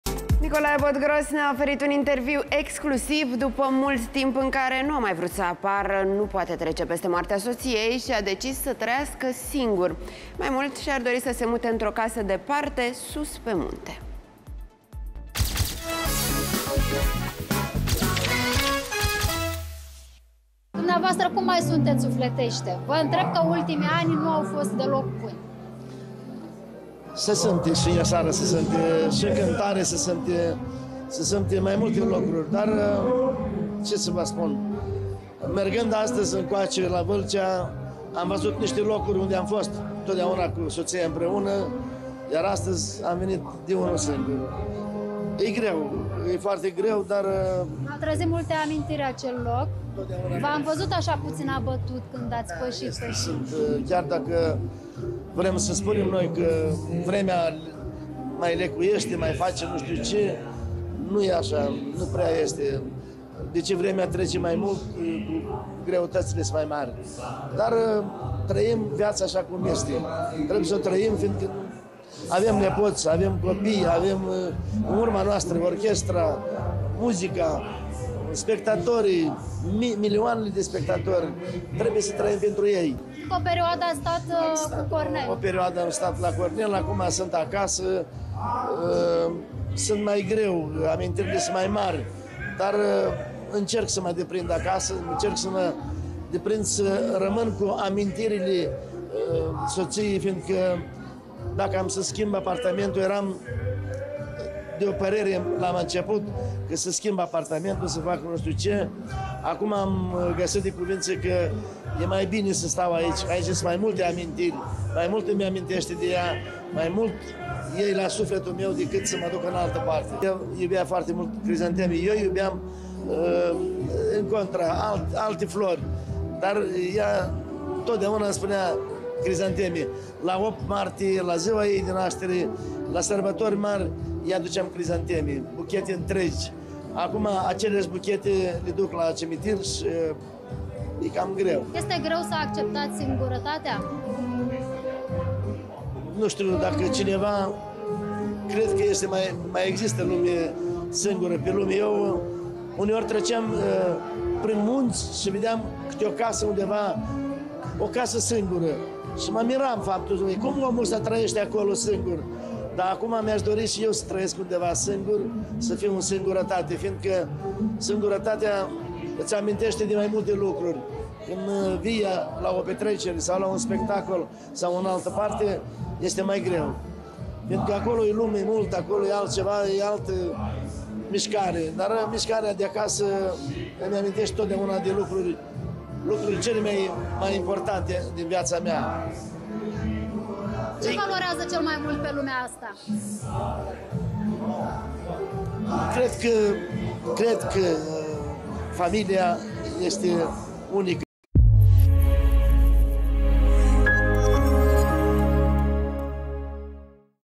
Interviu exclusiv! Nicolae Botgros vrea să trăiască singur, după moartea soției
Nicolae BotGros a oferit un interviu exclusiv după mult timp în care nu a mai vrut să apară. Nu poate trece peste moartea soției și a decis să trăiască singur.